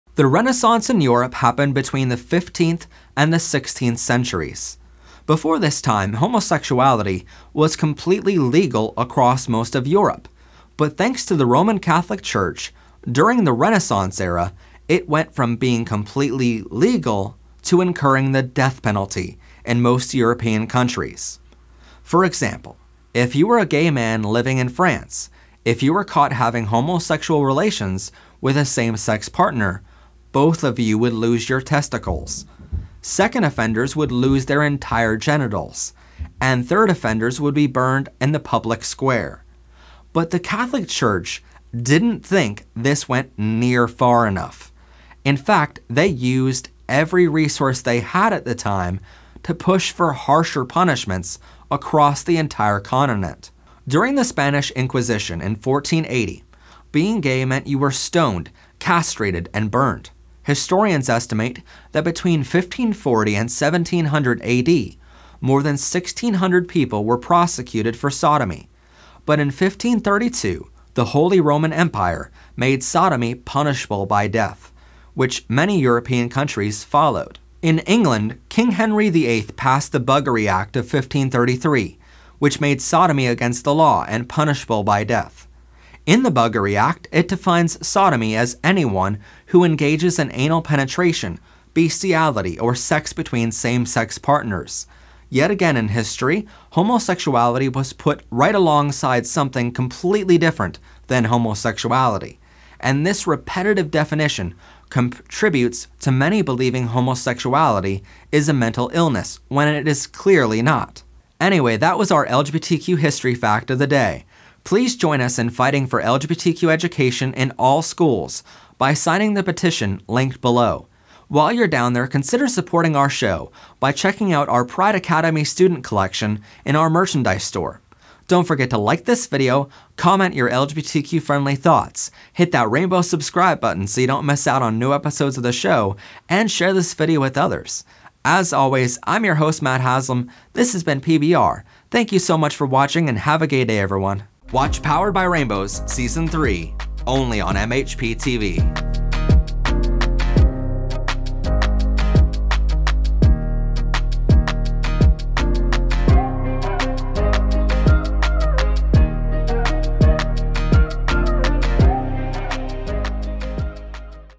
The Renaissance & The Buggery Act of 1533
During the Renaissance, homosexuality went from being legal to a capital punishment across most of Europe thanks to the Roman Catholic Church and the Buggery Act of 1533.